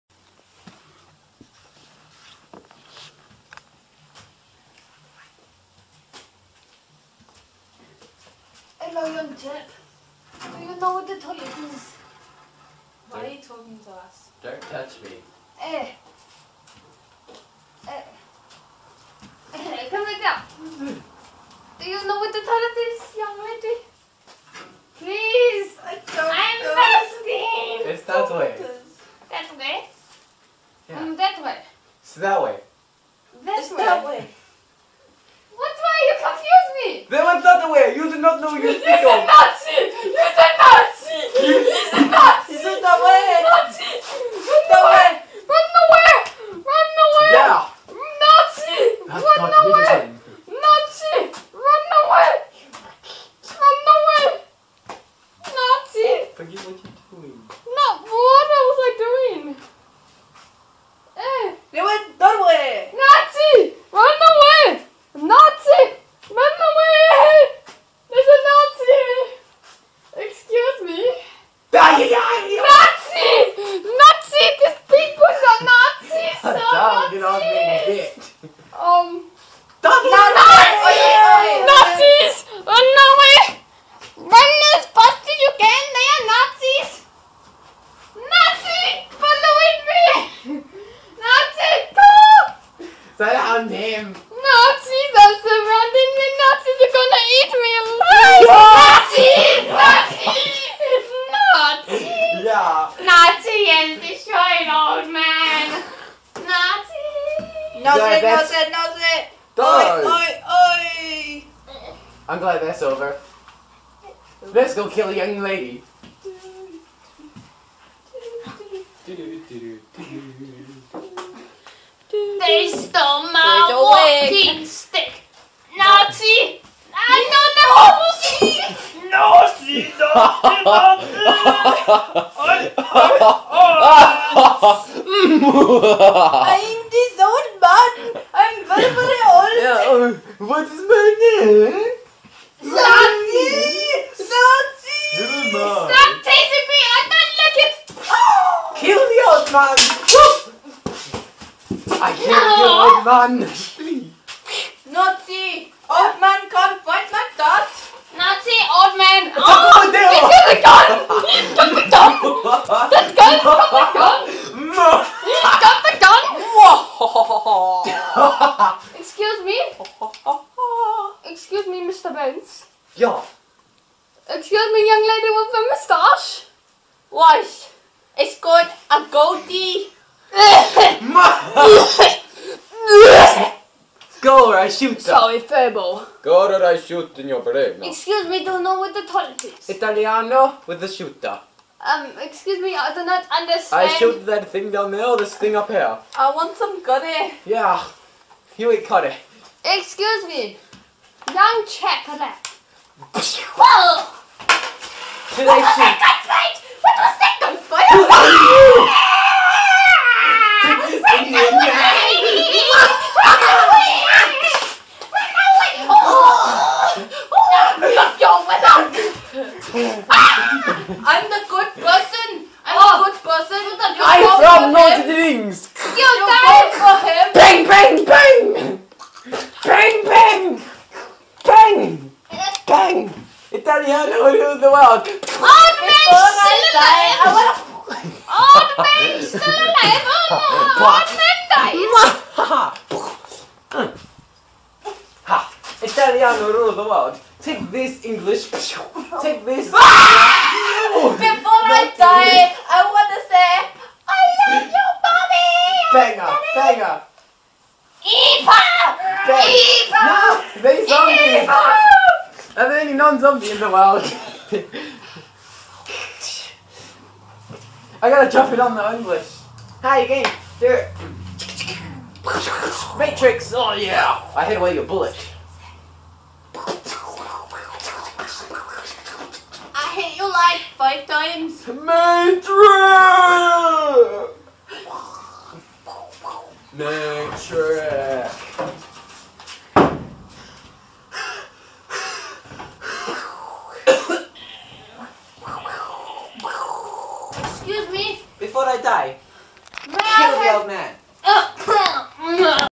The Mizzles - The Old Man
Something to pee your pants at... we were out of ideas lol so we did something random, no script.